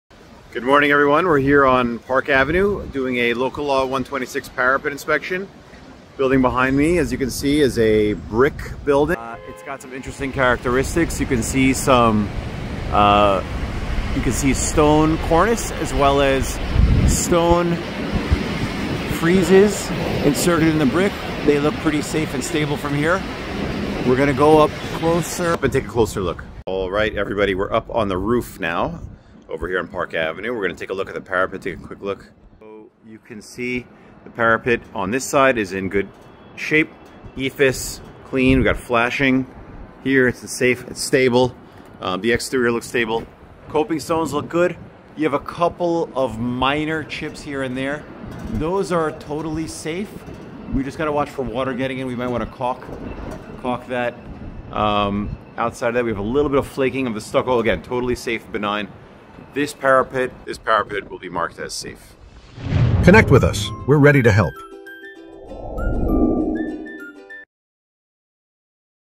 What We Found Inspecting a Park Avenue Parapet in NYC | NY Engineering
Curious about NY Engineering services or a potential project? We'd love to hear from you.